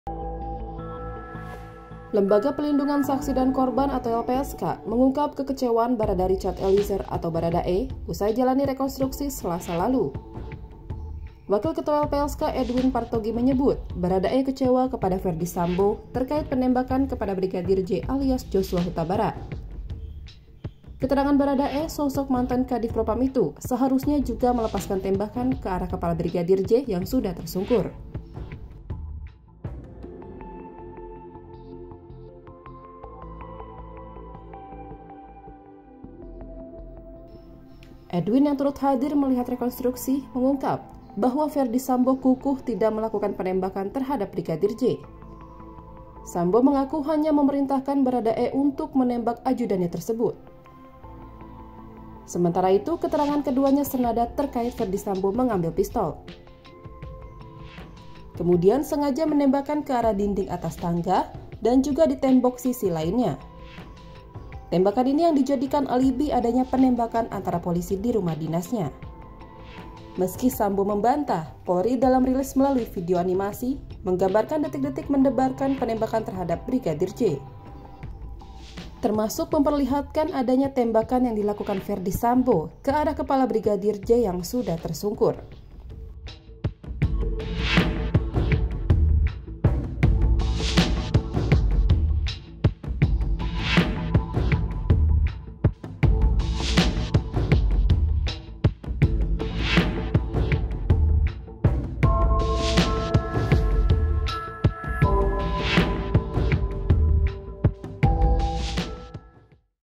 Bharada E Kecewa, Ferdy Sambo Tak Akui Tembak Kepala Brigadir J saat Rekonstruksi
MERDEKA.COM - Lembaga Perlindungan Saksi dan Korban atau LPSK, mengungkap kekecewaan Bharada Richard Eliezer atau Bharada E, usai jalani Rekonstruksi, Selasa lalu.

Wakil Ketua LPSK Edwin Partogi menyebut, Bharada E kecewa kepada Ferdy Sambo, terkait penembakan kepada Brigadir J alias Yoshua Hutabarat. Keterangan Bharada E, sosok mantan Kadiv Propam itu seharusnya juga melepaskan tembakan ke arak kepala Brigadir J yang tersungkur.